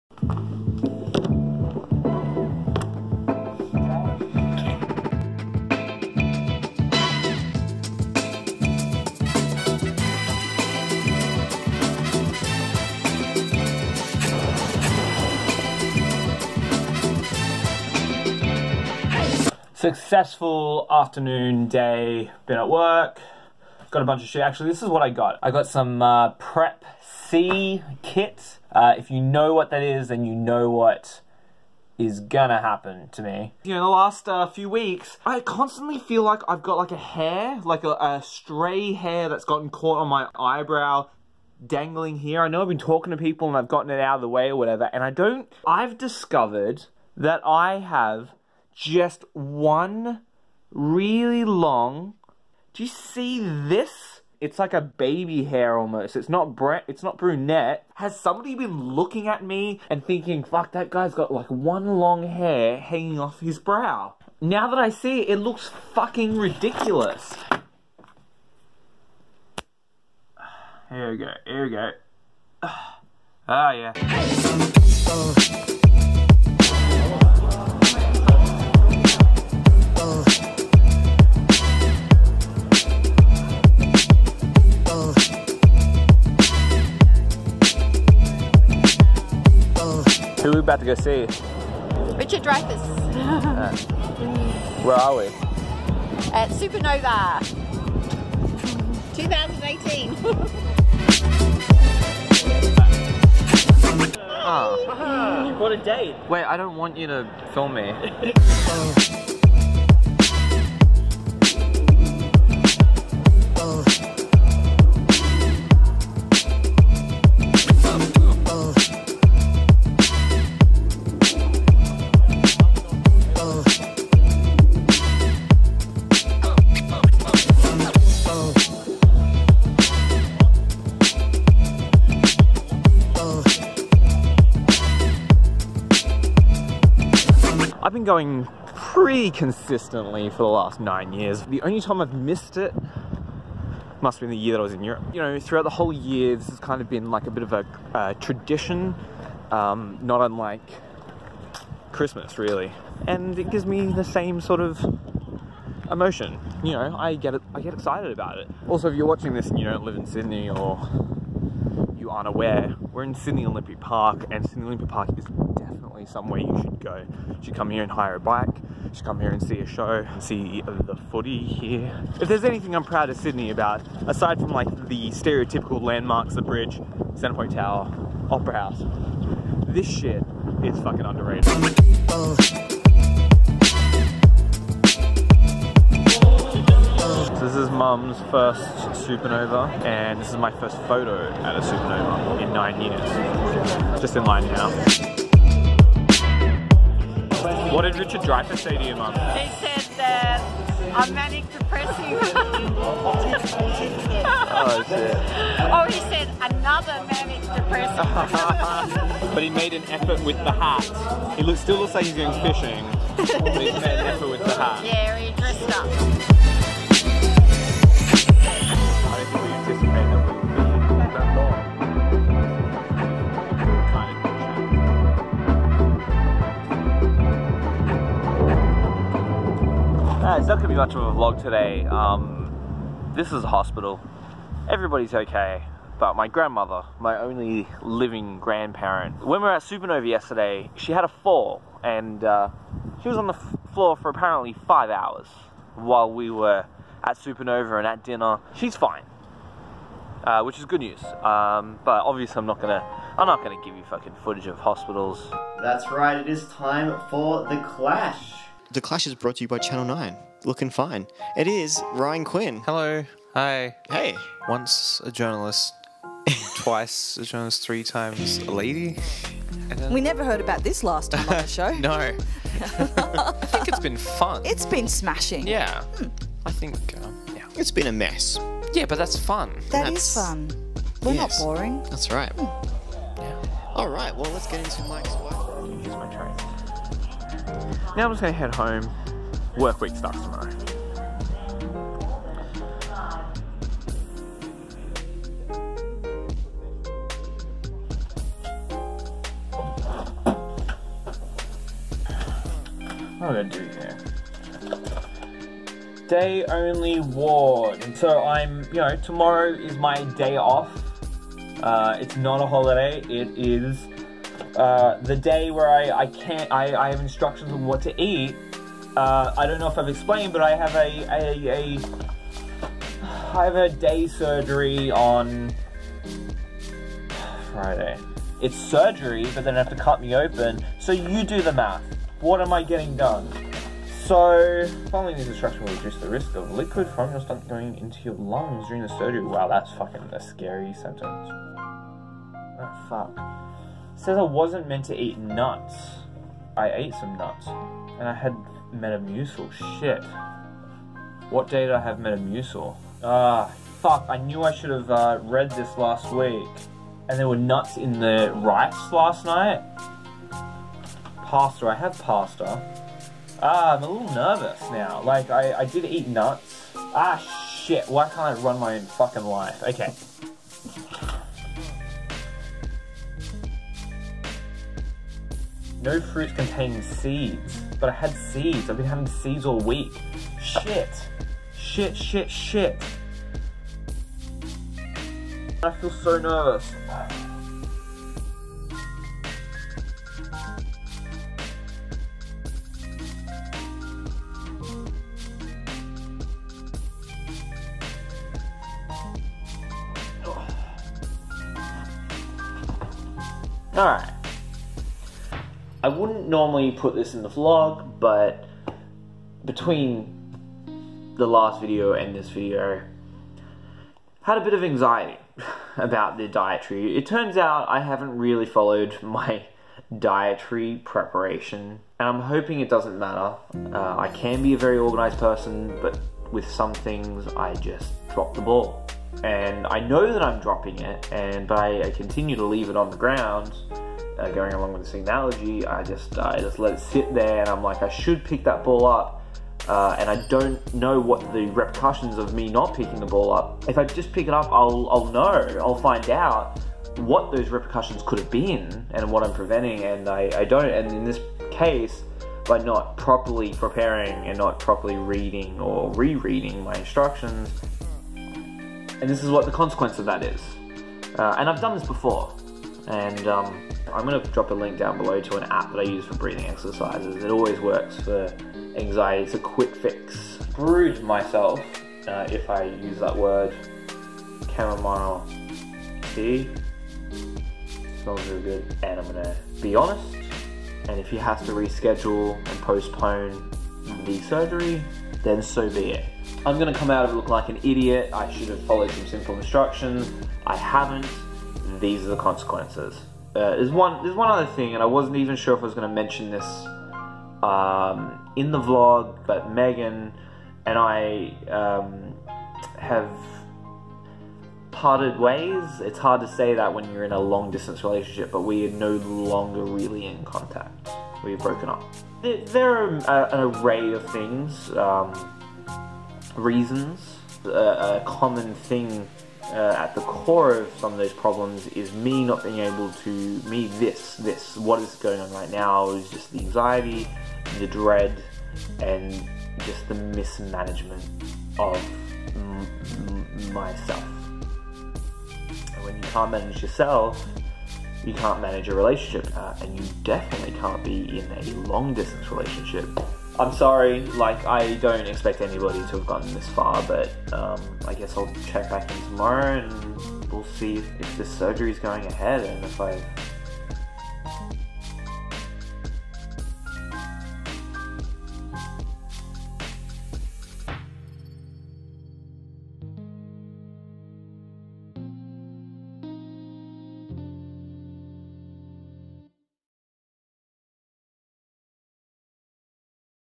#28 - Richard Dreyfuss called Mum a manic depressive
That breathing app is called "Paced Breathing", been using it sporadically for over a year. Android I'm sure there's a similar one for iOS out there.